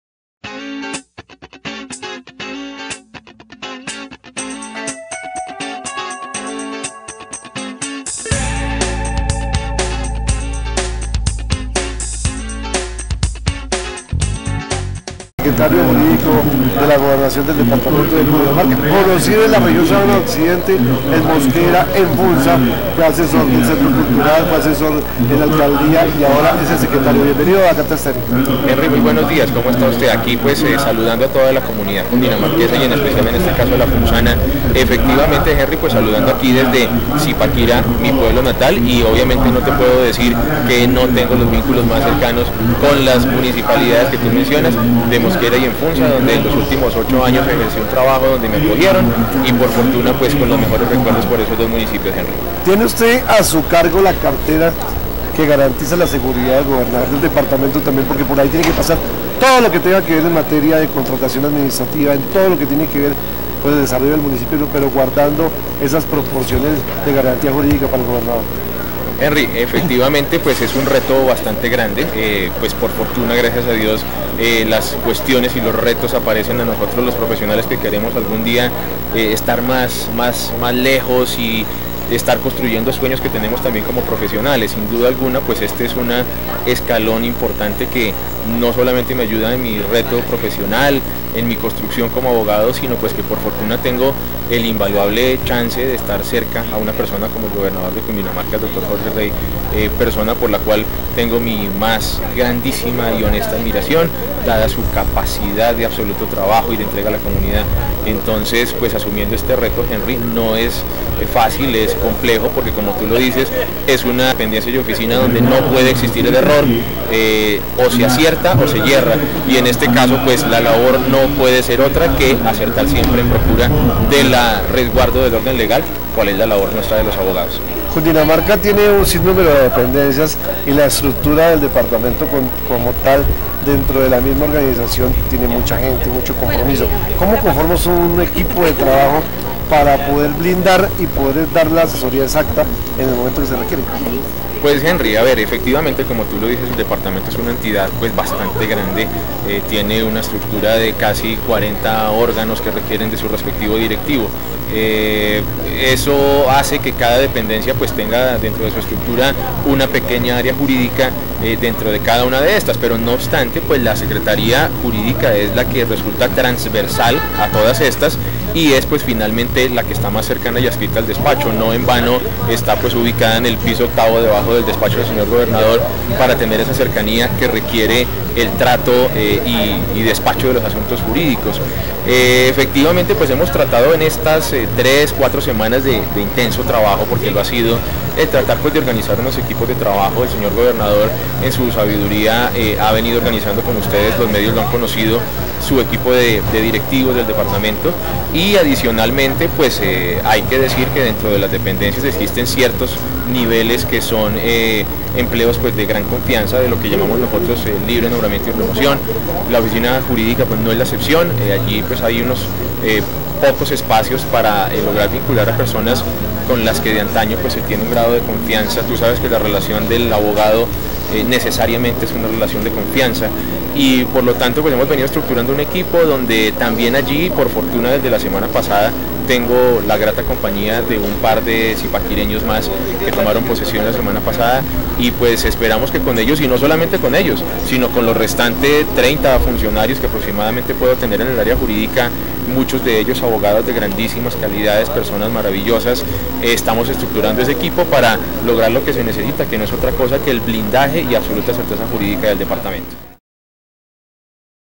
PORTAL DE NOTICIAS DE CUNDINAMARCA - 24 – 2016
Entrevista con German Enrique Gonzalez Secretario juridico de la Gobernacion de Cundinamarca